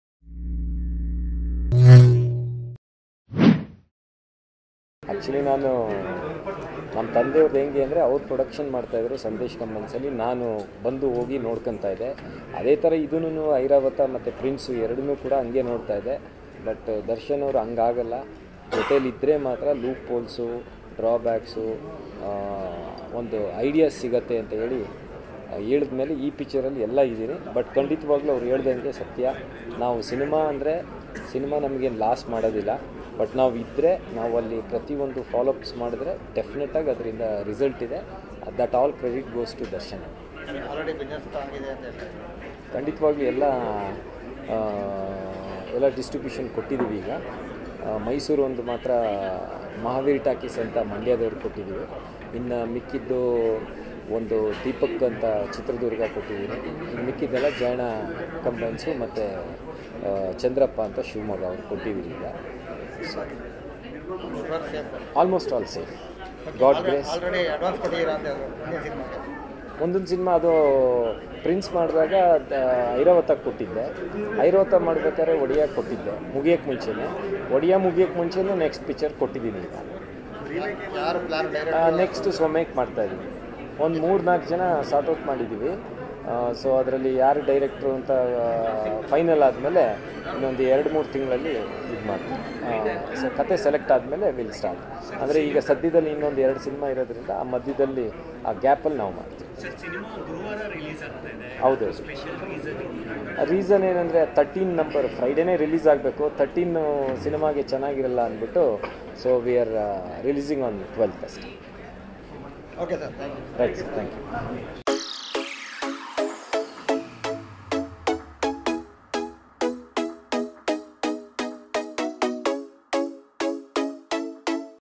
ದರ್ಶನ್ ಮುಂದಿನ ಸಿನಿಮಾ ಸ್ವಮೇಕ್ ಆಗಿರುತ್ತೆ, ಅದಕ್ಕೆ ನಾನೇ ಪ್ರೊಡ್ಯೂಸರ್ | Darshan Next Film Updates | D Boss
Odeya Movie Producer Sandesh N Speaks About D Boss Darshan Challenging Star Darshan Next Movie Updates New Films